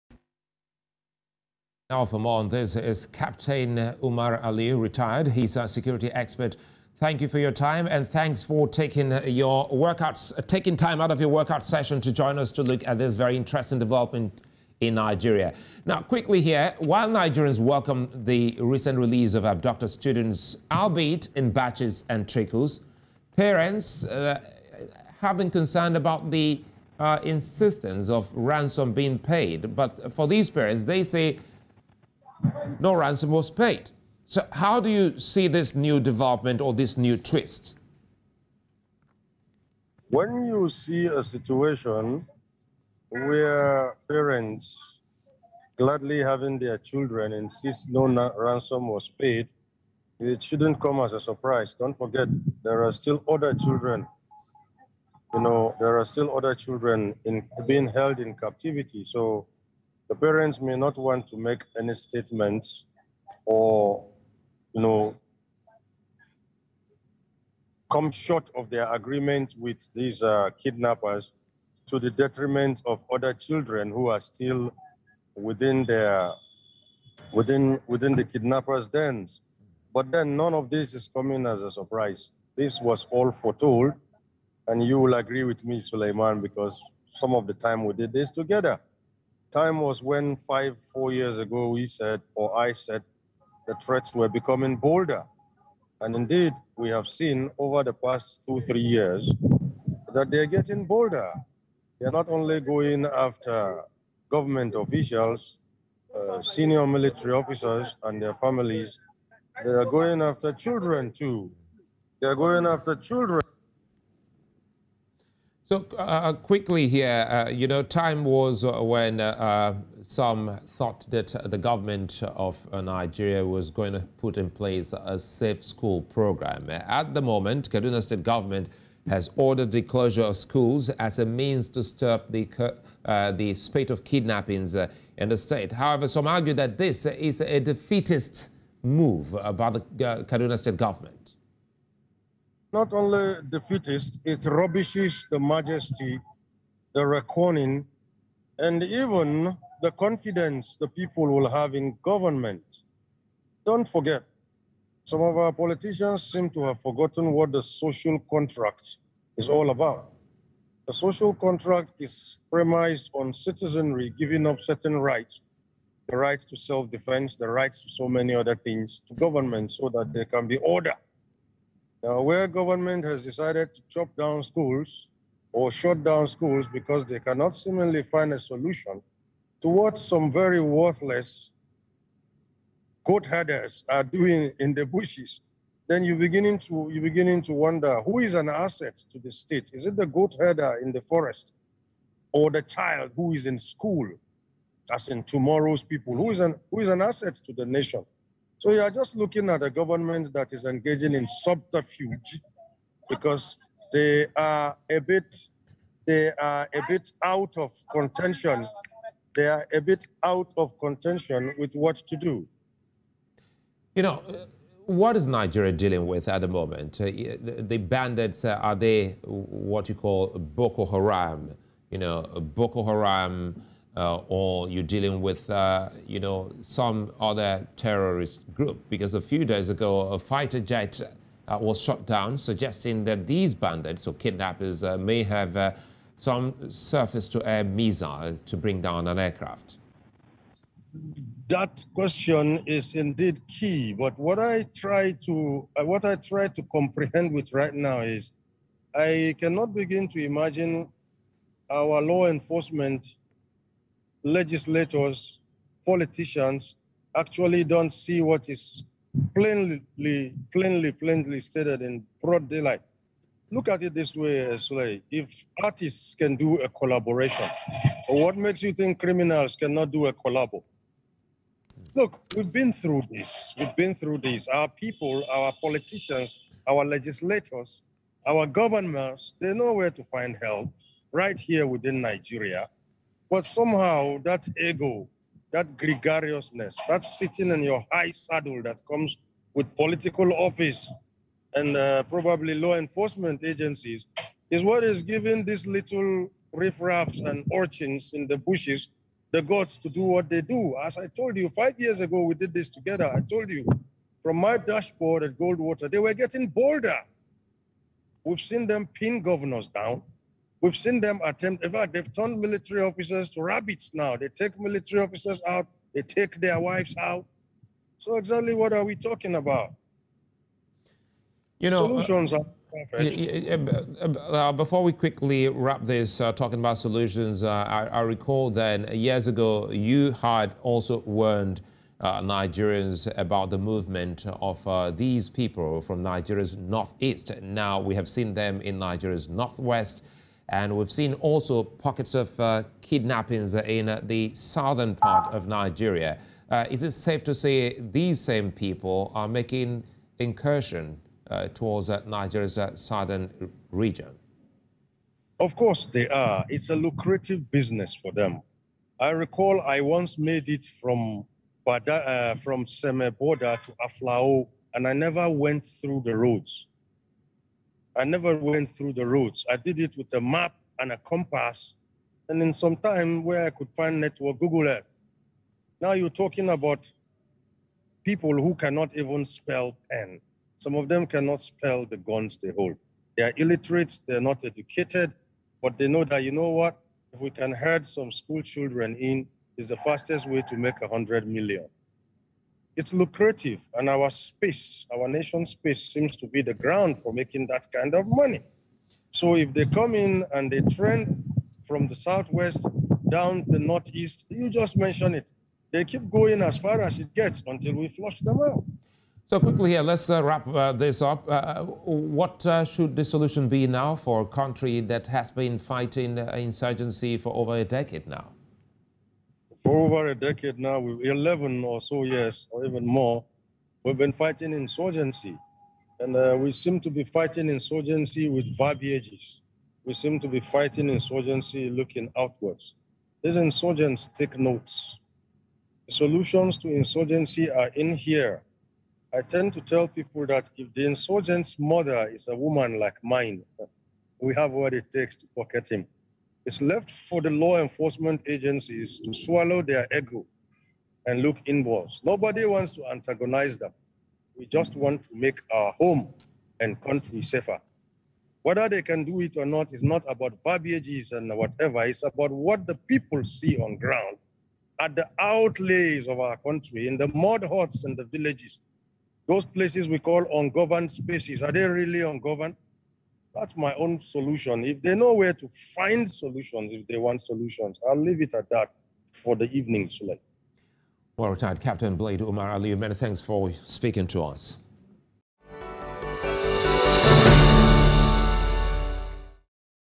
Nigeria Kidnappings: Bandits Shoot Down Fighter Jet// Kaduna Orders Closure of Schools
In West Africa, another four abducted students of Bethel Baptist High School in Nigeria's north-western state of Kaduna have regained their freedom.